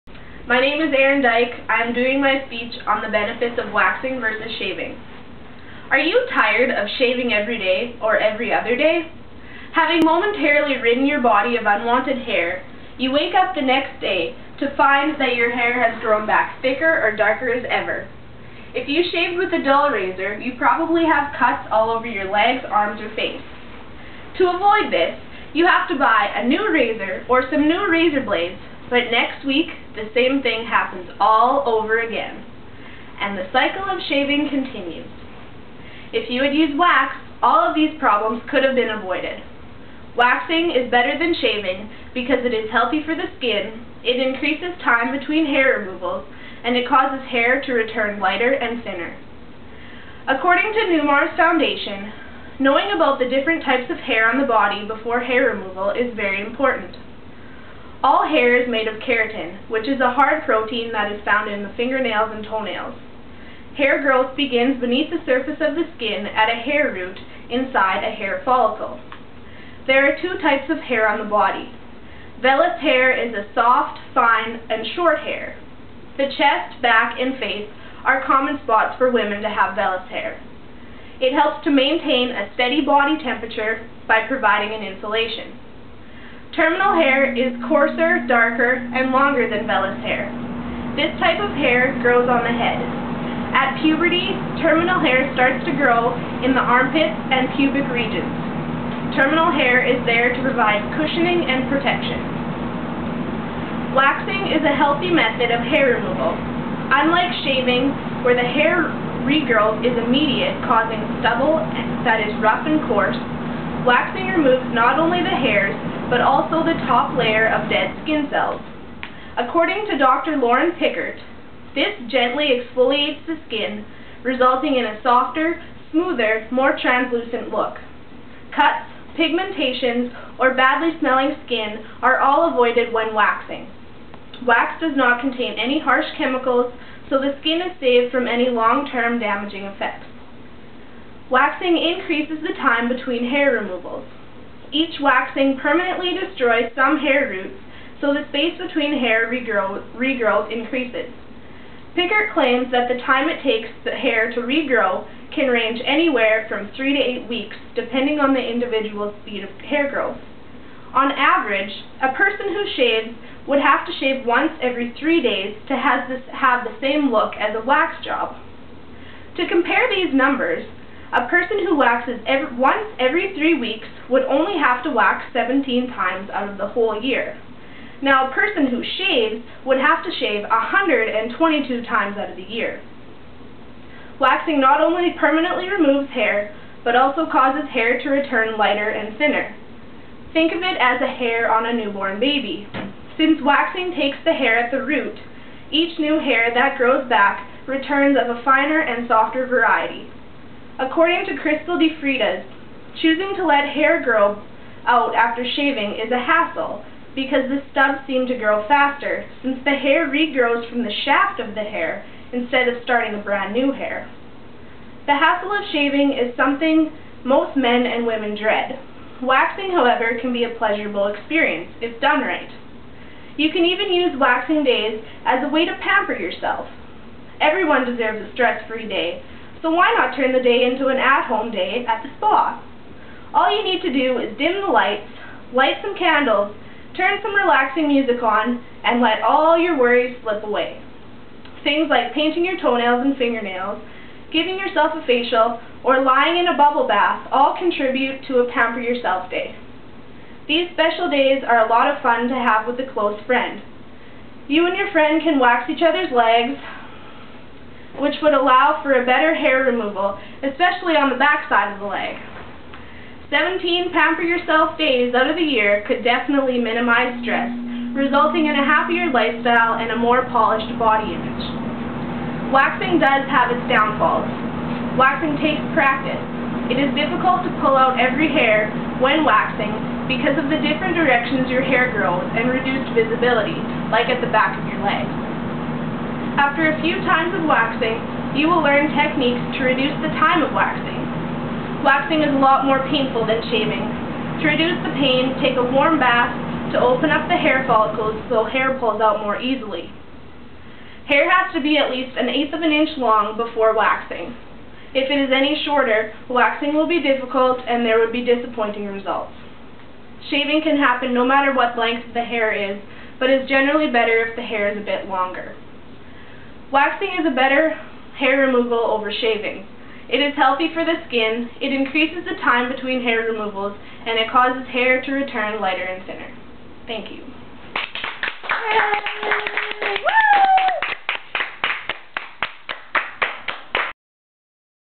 Picture 317.avi
Erin Dyok Persuasive Speech on waxing vs shaving